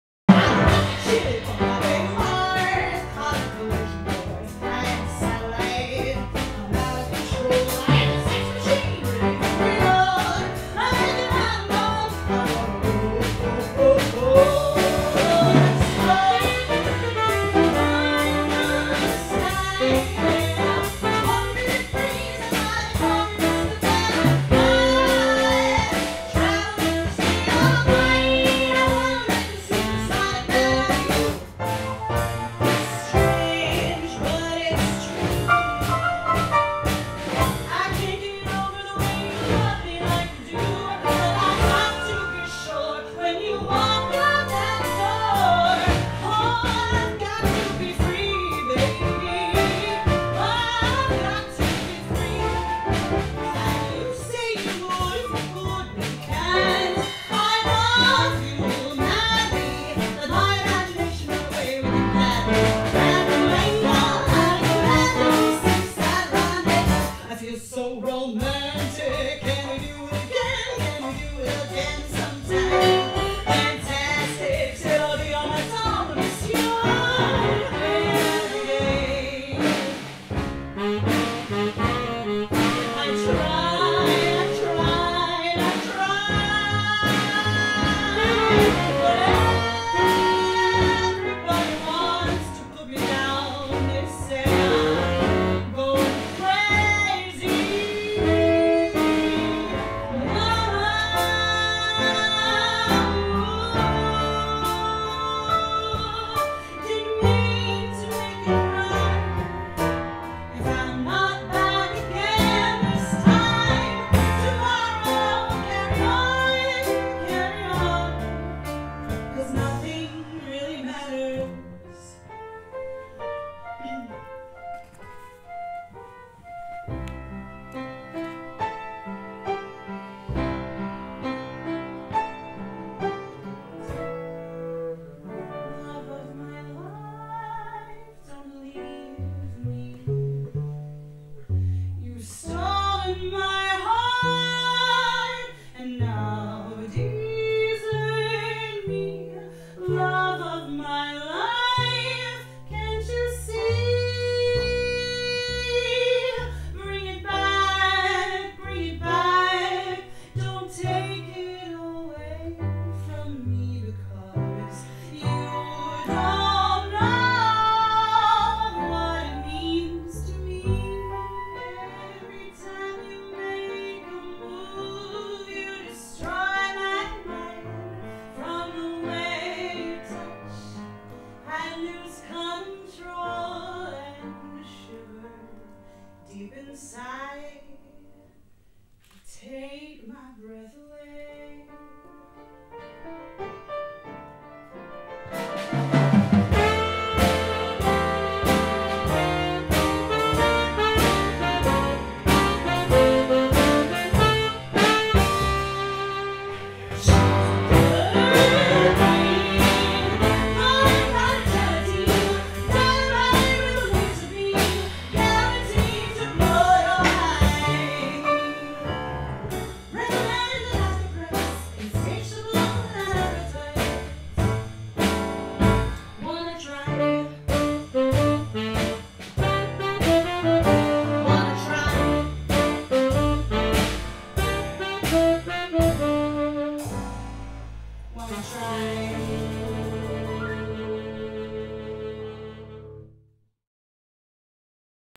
Mercury Rising Promo Video
Courtney Freed’s one-woman show "Mercury Rising" highlights Freddie Mercury's incredible range and dynamic stage presence through vocal driven jazz arrangements. Freed also brings to light the life experiences that shaped his talent and ultimately led to his untimely AIDS related death in 1991. "Mercury Rising"  is not simply a concert of Queen’s music, but rather an homage paid to Mercury through the art of cabaret storytelling and a fresh musical take on his most beloved songs.

Directed by Isaac Lamb and originally produced by Live on Stage Productions at Coho Theater on April 4-8, 2018.

Arrangements by Reece Marshburn
David Saffert on piano
Tom Goicochea on drums
Josh Gilbert on sax, flute and uke
Bernardo Gomez on bass

Shot and edited by Steve Brian